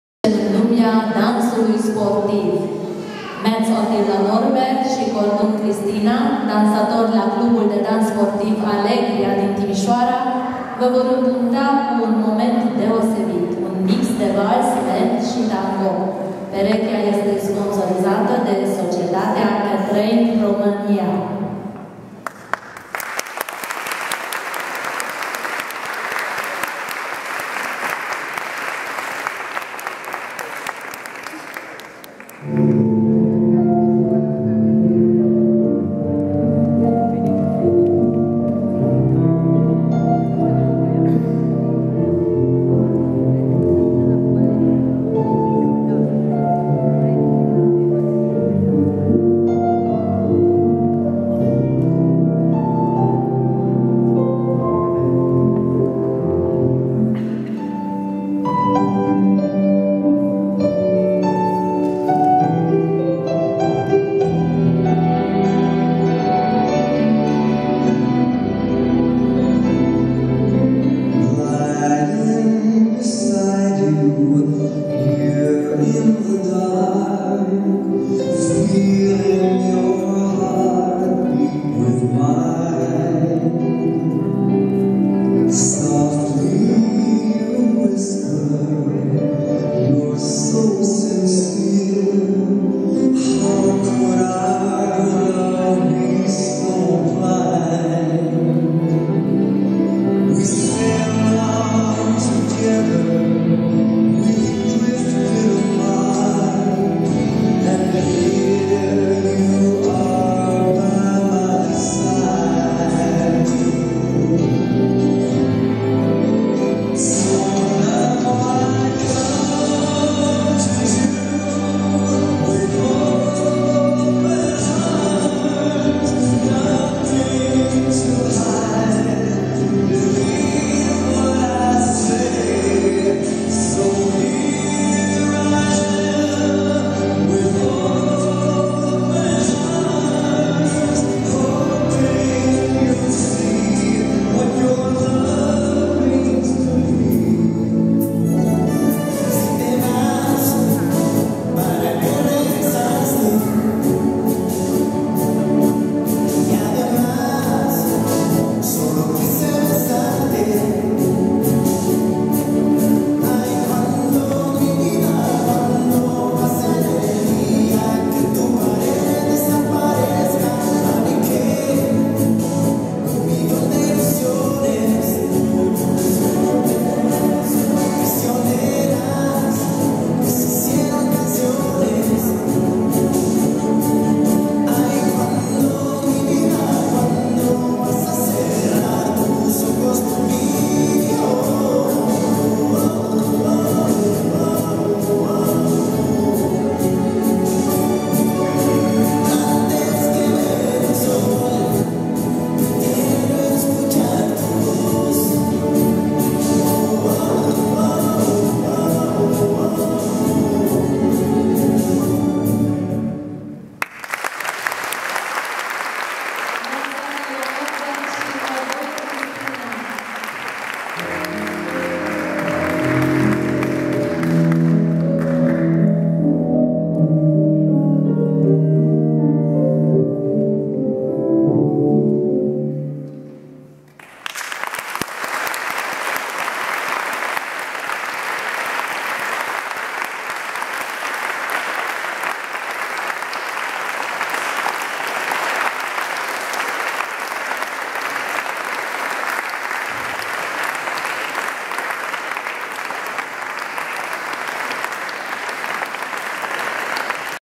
2013-11-09 CBT 040 - Demo A.N. Metz & C. Cordun (Alegria, Timisoara) - Vals si Tango
RO: "Cupa Ballroom Timisoara" Editia I - Show - Demonstratie dans sportiv - Atilla Norbert Metz si Cristina Cordun de la clubul de dans "Alegria", Timisoara (instructor: Vitalie Trohin) - Vals si Tango

EN: "Cupa Ballroom Timisoara" Dance Competition, 1st Edition - Show - Dancesport Demo - Atilla Norbert Metz & Cristina Cordun from "Alegria" Timisoara dance club (instructor: Vitalie Trohin) - Waltz and Tango

(Canon S110)